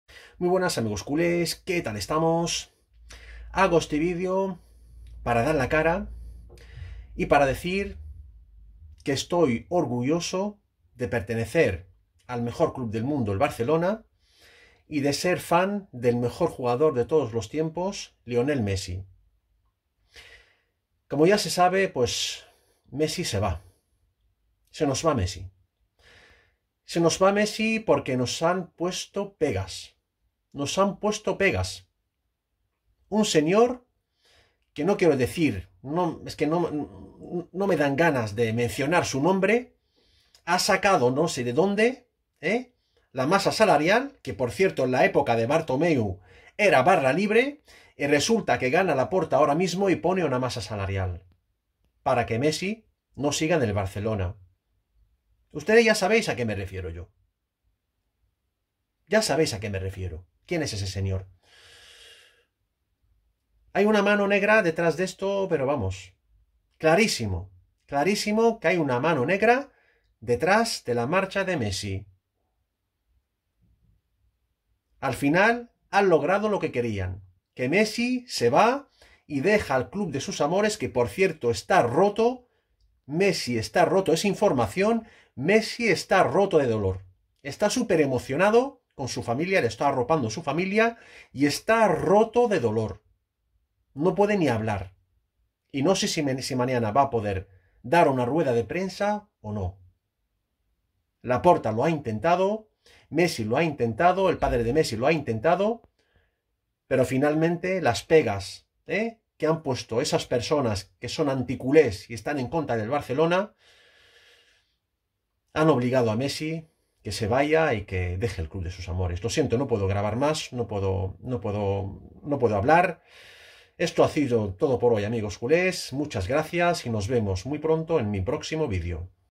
ÚLTIMA HORA ¡¡MESSI ACABA DE DESPEDIRSE DEL BARCELONA ROTO DE DOLOR Y LLORANDO!!
Messi se despide del Barcelona roto de dolor y entre lágrimas